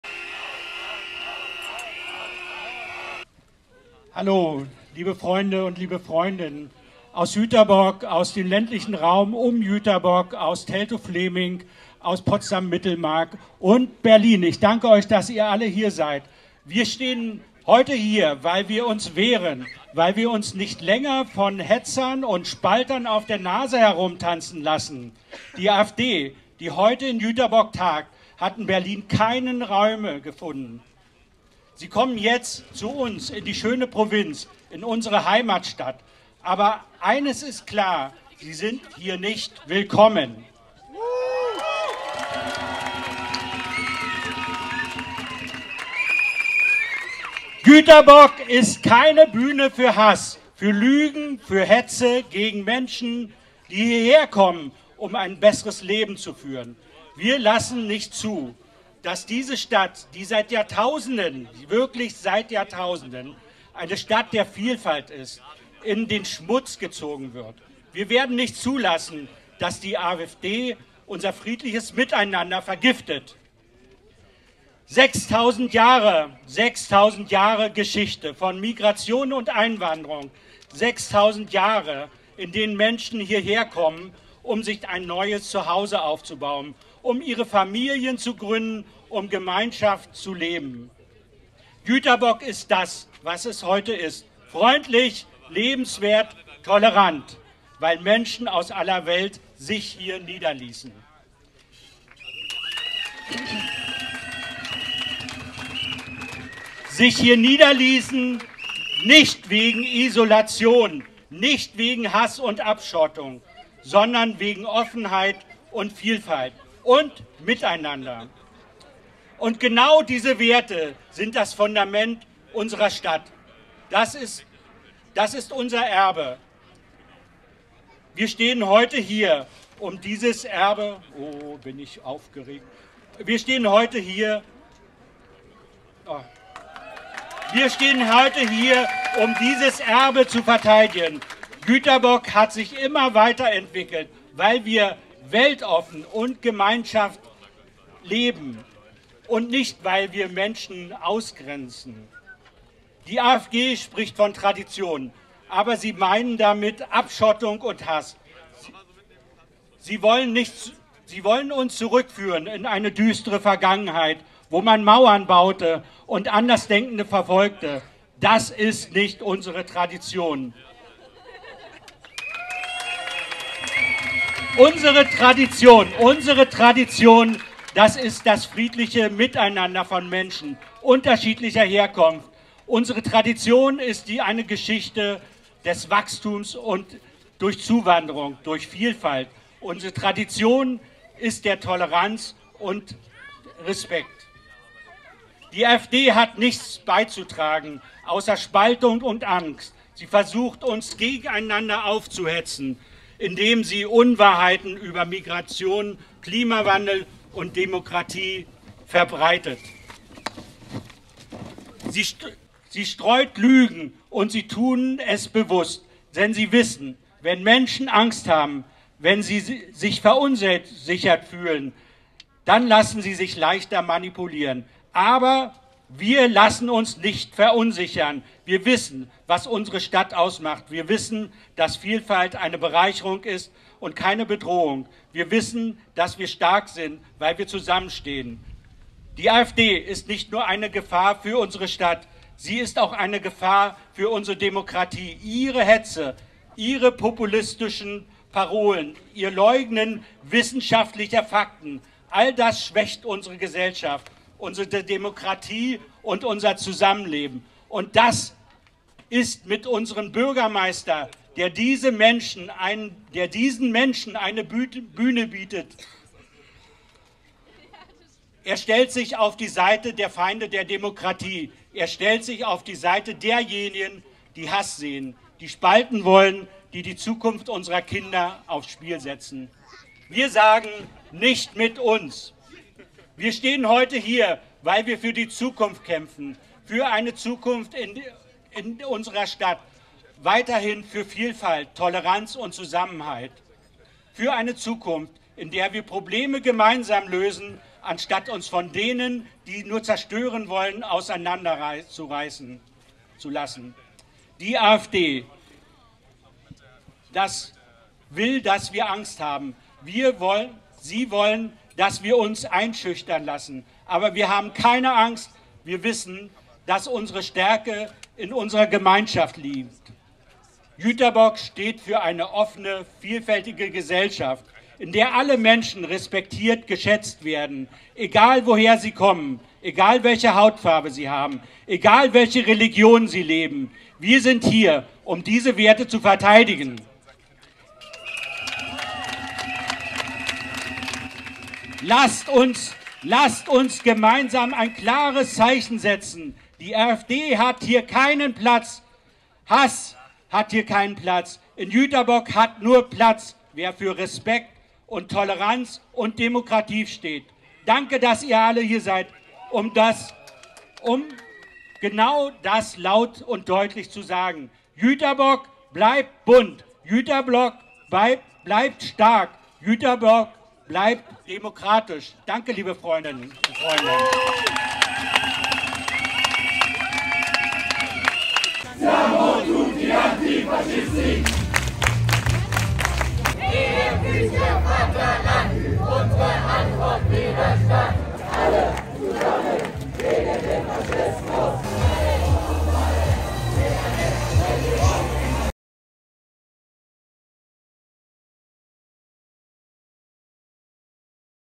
Kein Platz der AfD! Offenheit, Vielfalt, Miteinander verteidigen! (K.-P. Gust, Grüne Niedergörsdorf)
Jüterbog (Brandenburg) bei Berlin, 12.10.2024